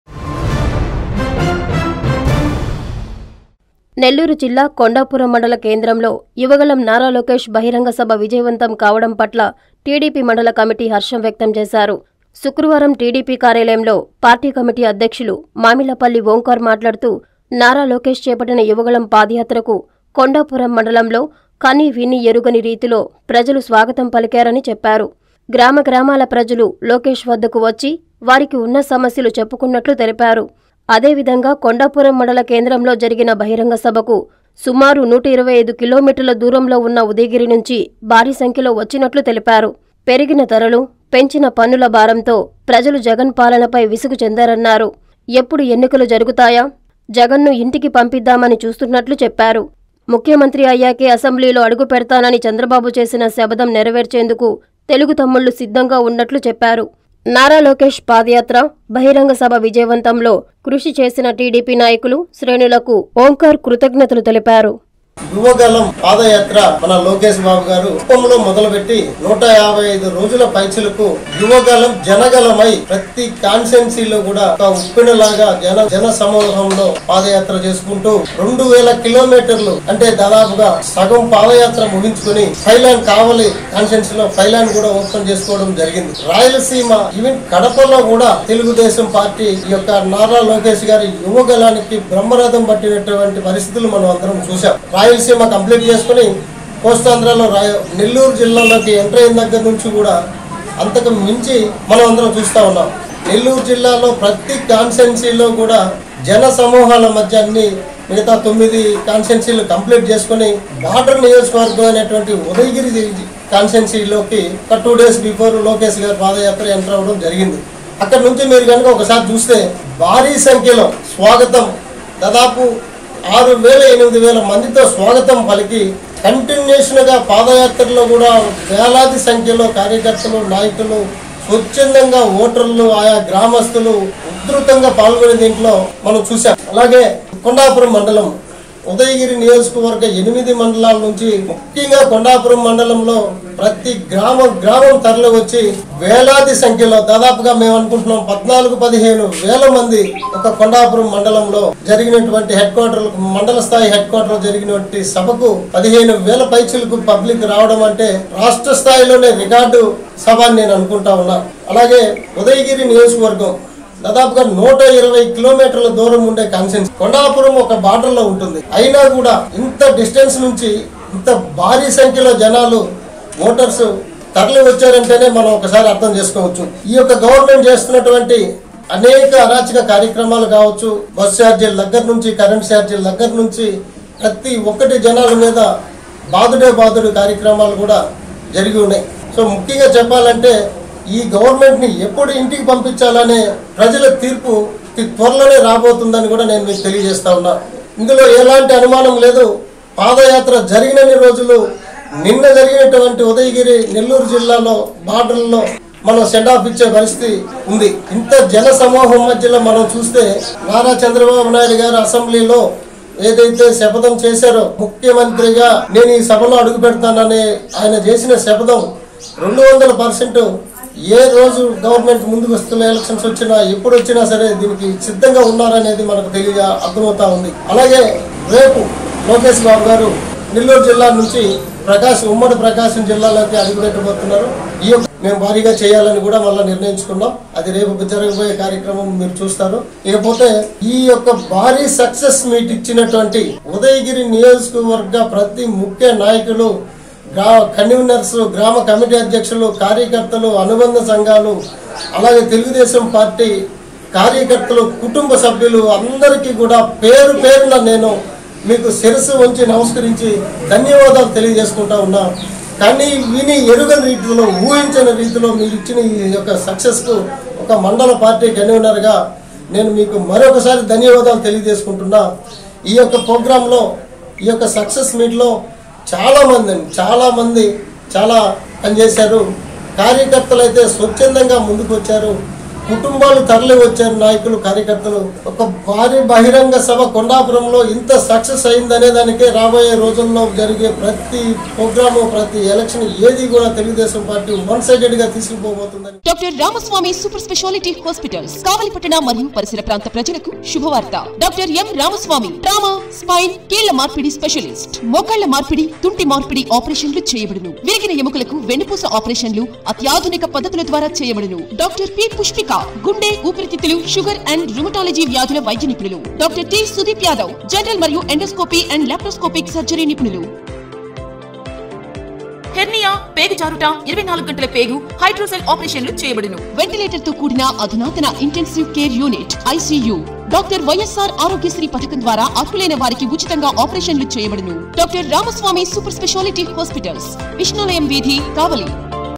చంద్రబాబు శప‌దాన్ని నెరవేర్చుతాం...! | Lokesh Yuvagalam Sucess Meet
కొండాపురంలో లోకేష్ బహిరంగసభ విజయవంతం పట్ల  టీడీపీ మండల కమిటీ హర్షం
ఇందుకు సహకరించిన టీడీపీ నాయకులు, శ్రేణులకు కృతజ్ఞతలు